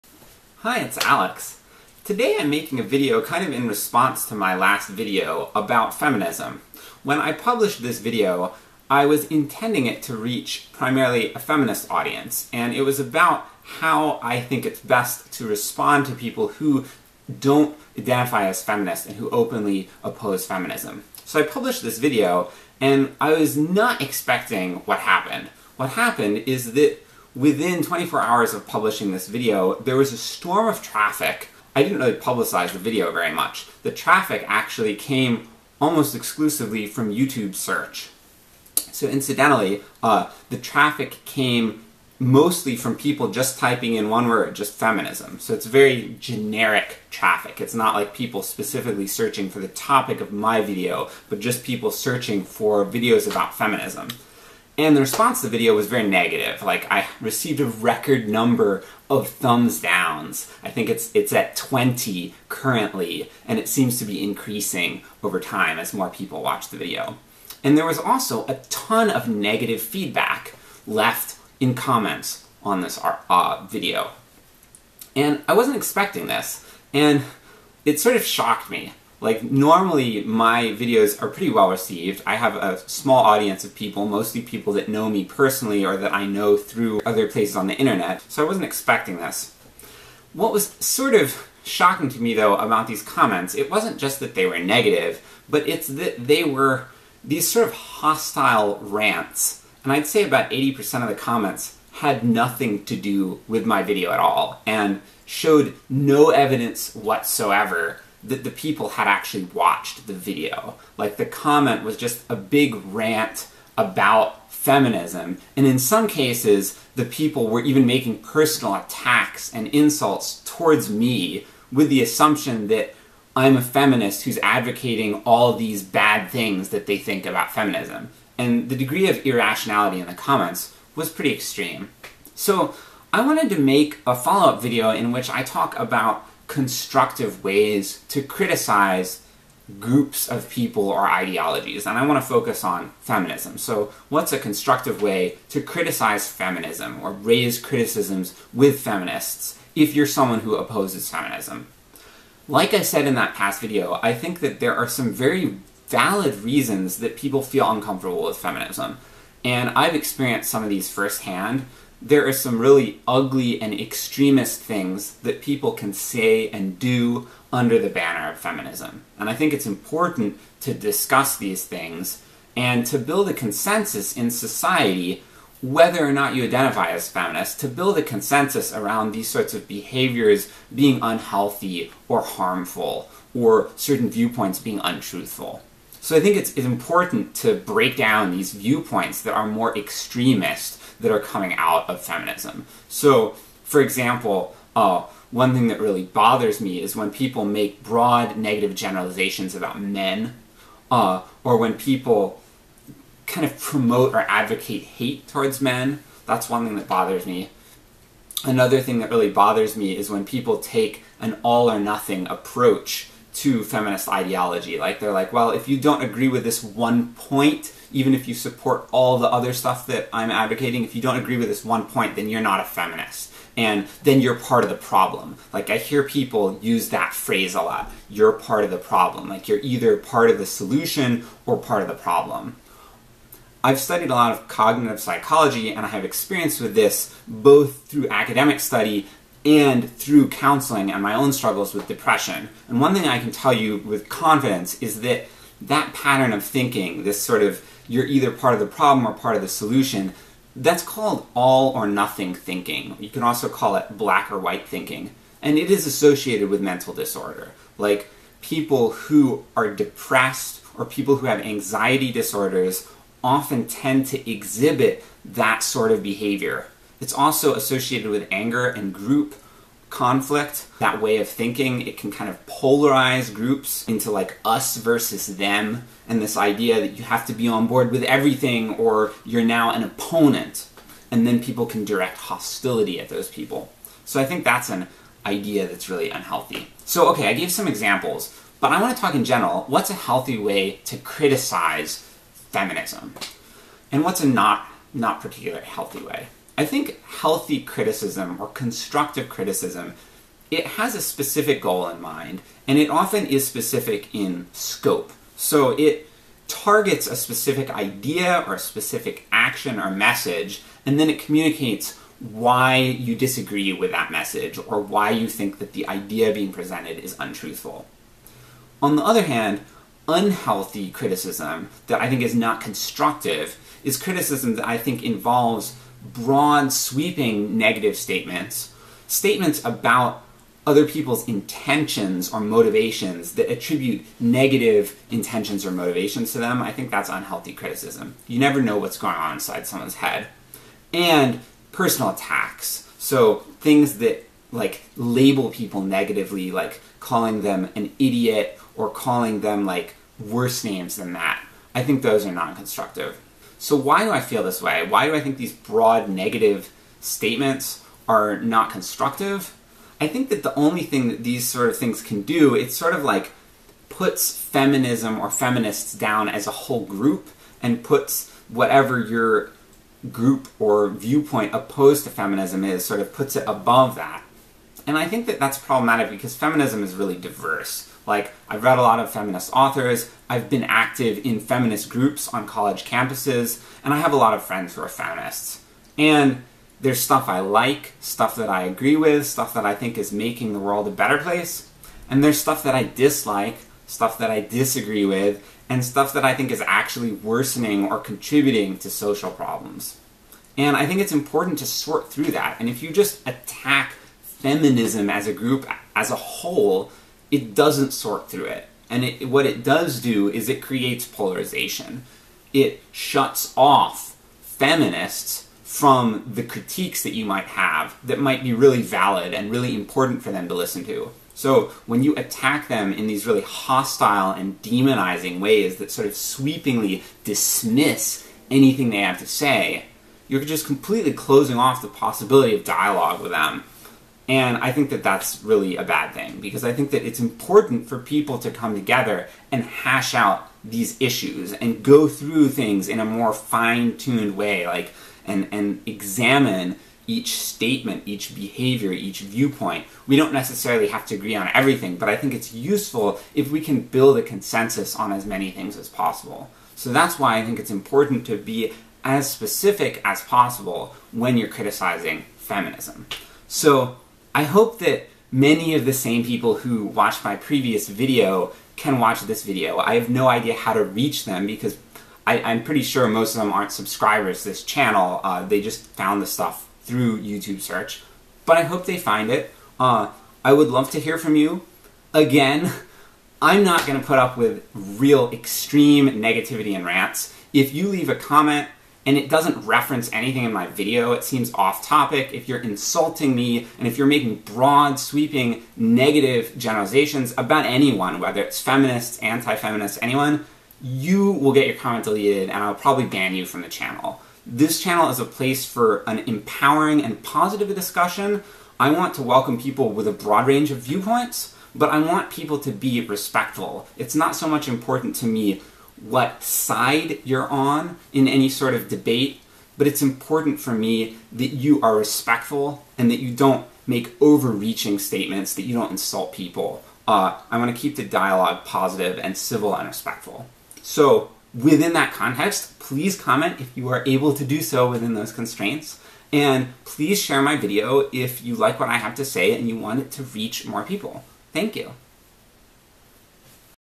Constructive Ways To Criticize Feminism
My previous video attracted a huge amount of irrational rants about feminism from people, most of whom didn't engage with anything I said at all. I talk about this experience, and explain what I think of as a healthier and more constructive way to share criticism of feminism with feminists.